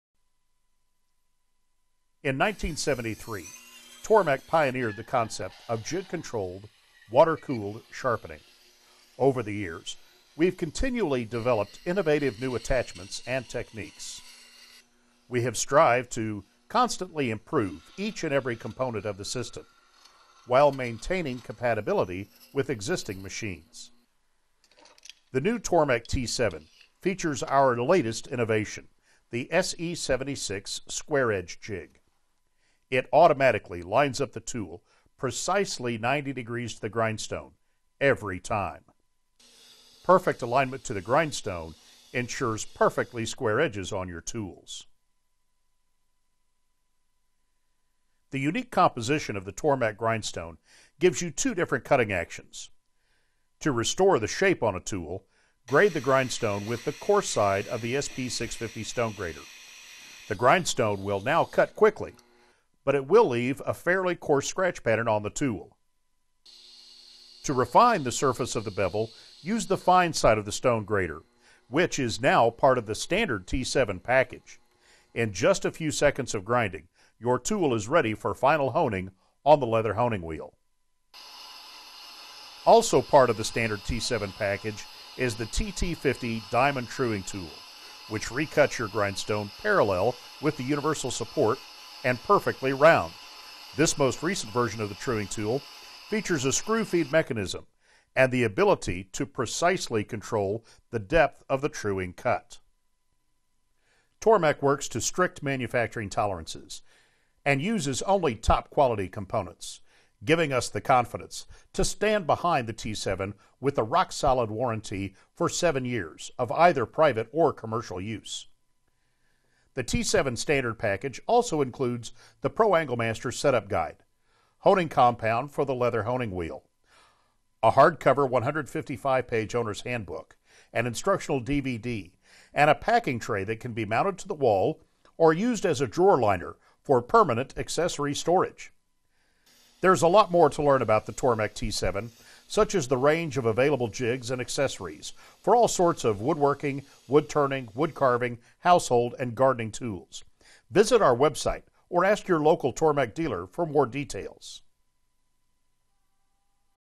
Tormek T7 | Knife Grinder | LOGOSOL
For sharpening planing and moulding knives and other tools. 220 V (or 120 V). Comes with a very informative, hardback instruction book of 147 pages.

Keep sharp with the Tormek method
The grinding stone revolves slowly, no more than around 100 rpm, dipping into a water bath as it does so. It has everything needed to ensure perfect sharpening! All jigs and accessories fit both the T7 and T3. We recommend the Tormek T7 if you need to sharpen HSS steel for Logosol's machines, as its bigger motor can deal with higher grinding pressures. Both models have high quality motors and shafts made of stainless steel. T3 has a strong plastic stand, and the stand for the T7 is made of steel plate.